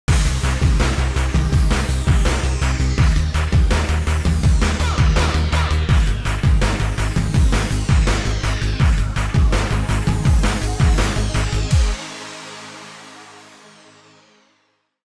Axient Digital - Bienvenido al futuro de la tecnología inalámbrica
Bienvenido al futuro de la tecnología inalámbrica. 184MHz de ancho de banda en transmisores y receptores, transmisión digital con máxima calidad de sonido manteniendo el RF más sólido jamás logrado en el mercado, Quadversity (4 antenas), modo de alta densidad, detección y evasión de interferencias propio de Axient, ShowLink, 2ms de latencia, 120 dB de rango dinámico, salidas digitales Dante, AES3 y análogas, micro-transmisor de cuerpo y mucho, mucho más. AxientDigital Integridad que se hace oír.